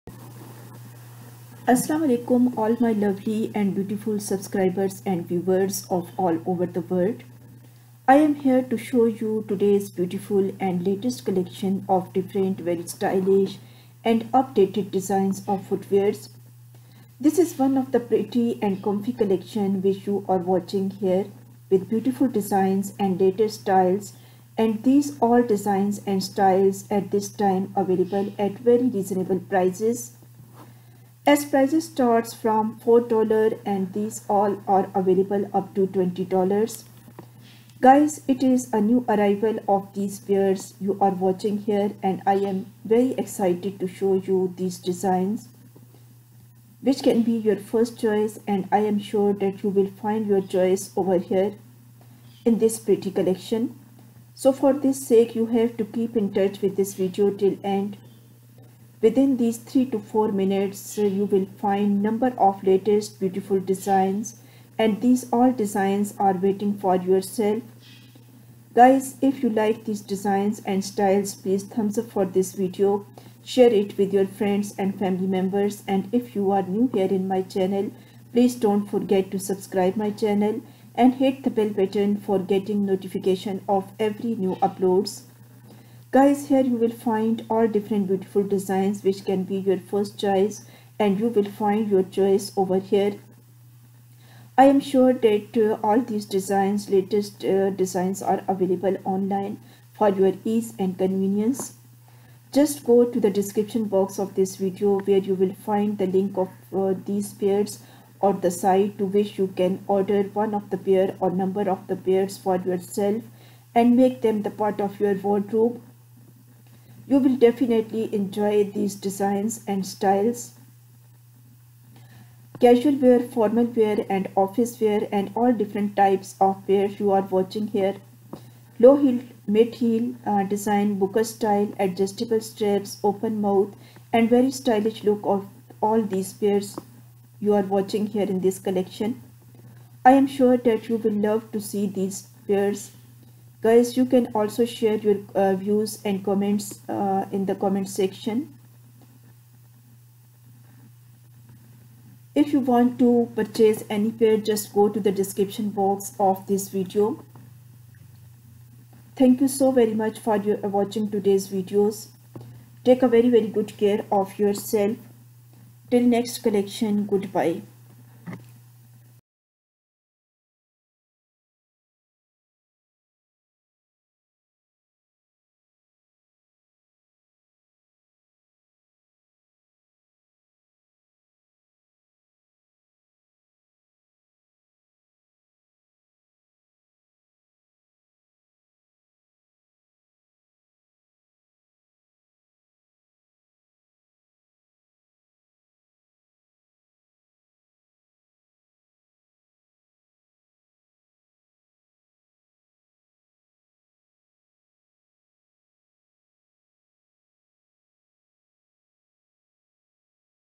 2023 OFFICE STYLE NEW LATEST SANDAL GENUINE LEATHER SANDALS OF SLIP ON SHOES DESIGN STYLISH COMFY
2023 OFFICE STYLE NEW LATEST SANDAL GENUINE LEATHER SANDALS OF SLIP ON SHOES DESIGN STYLISH COMFY
IN THIS VIDEO YOU CAN SEE LADIES FOOTWEARS.THESE ARE NEW DESIGNS OF SHOES.